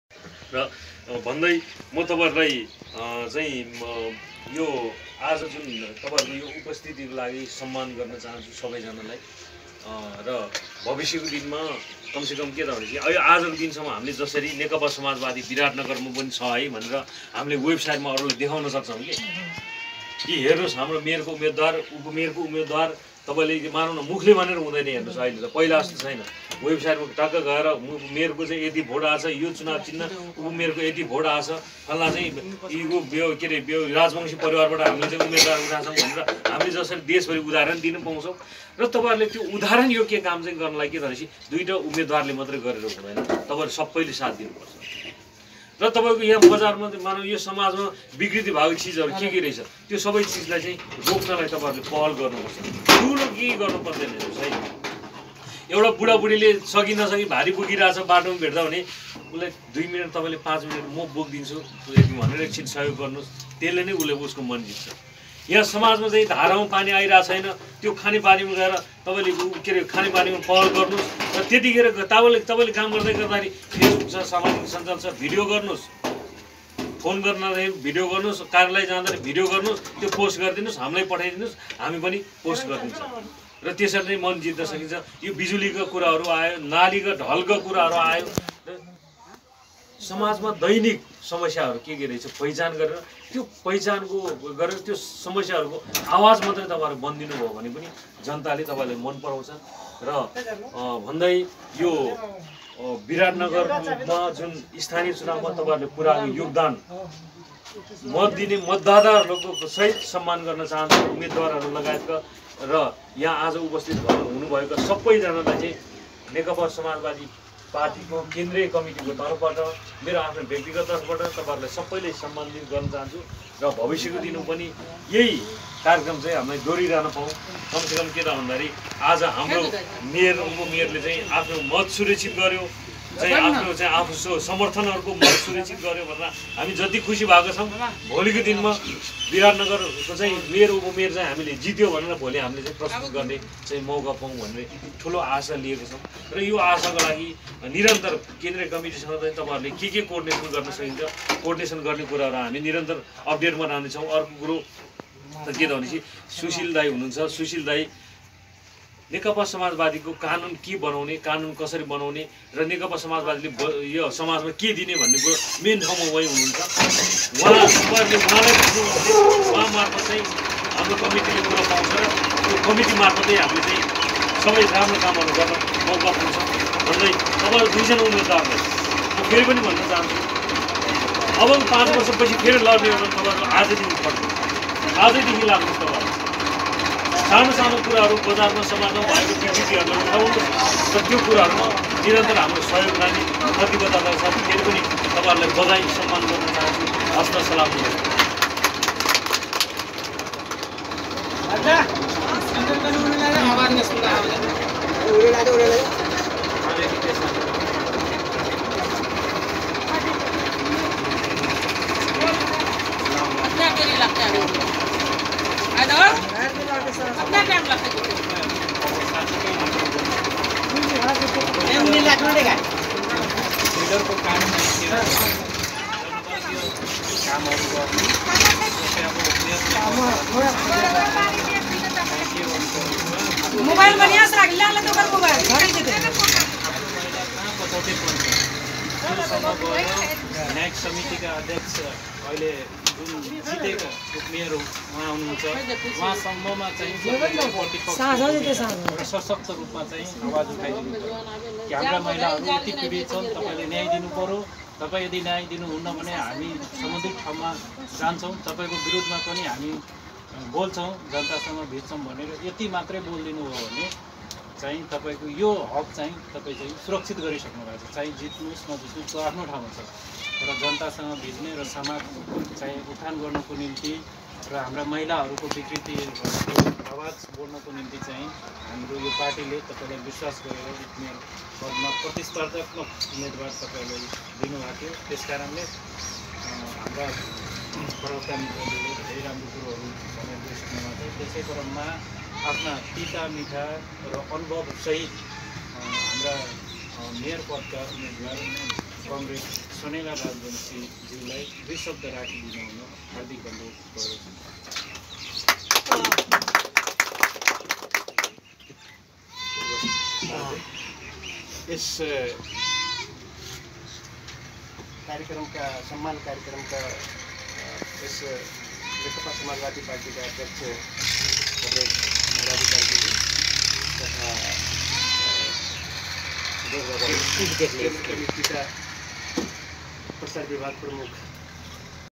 NCP Socialist Biratnagar | नेकपा समाजबादी पार्टी बिराटनगर
NCP Socialist

नेकपा समाजबादी पार्टी बिराटनगर

नेकपा समाजबादी पार्टी बिराटनगर अन्तर्गत स्थानिय निकयको उम्मेदवारहरुलाई सम्मान समिक्ष कार्यक्रम

NCP Socialist | Nepal Communist Party Socialist
Nepal Communist Party Socialist | नेकपा समाजबादी पार्टी बिराटनगर

राजु कार्की ः अध्यक्ष  (नेकपा समाजबादी पार्टी)
मान बहादुर कार्की ः प्रवक्ता (नेकपा समाजबादी पार्टी)
सुशिल कुमार राजबंशी ः सचिवालय सदस्य (नेकपा समाजबादी पार्टी)
सोनेलाल राजबंशी ः बिराटनगर मेयर (नेकपा समाजबादी पार्टी)
संदिप थापामगरः प्रदेश १ इन्चार्ज (नेकपा समाजबादी पार्टी)
लक्ष्मि राजबंशी ः बिराटनगर उपमेयर (नेकपा समाजबादी पार्टी)
तथा
सम्पुर्ण मोरगं र सुनसरी जिल्ला सदस्यहरु